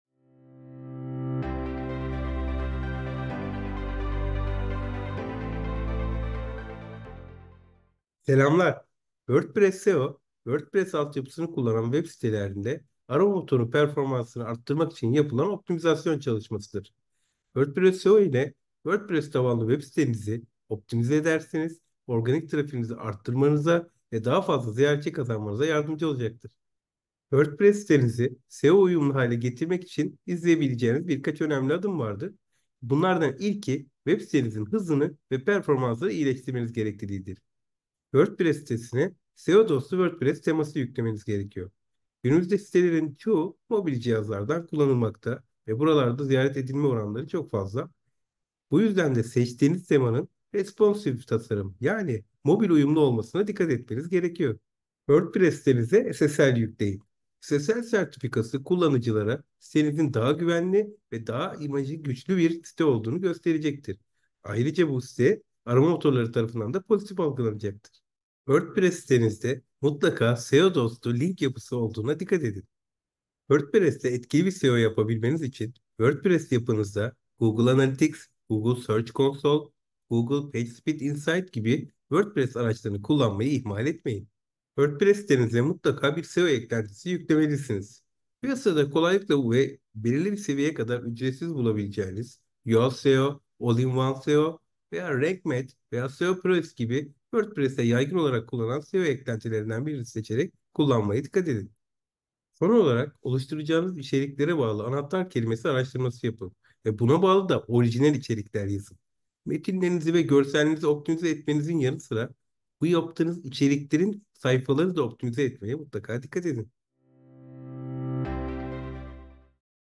WordPress SEO Nedir? | Aytaç Mestçi
WordPress SEO, WordPress alt yapısı kullanan web sitelerinde arama motoru performansını arttırmak için yapılan optimizasyon çalışmalarıdır.

👉 İşte, WordPress sitenizi SEO uyumlu hale getirmek için izleyebileceğiniz birkaç adım…

00:00 WordPress SEO Nedir?
00:32 WordPress ile Web sitenizin hızını ve performansını iyileştirme
00:38 SEO dostu bir WordPress teması yükleme
00:55 Wordpress SSL sertifikası yükleme
01:08 Wordpresste SEO dostu link yapısı kullanma
01:13 Wordpress Google Analytics, Google Search Console, Google Pagespeed Insights gibi araçları kullanma
01:25 WordPress SEO eklentisi yükleme

📌 Sosyal Ağ Hesapları ►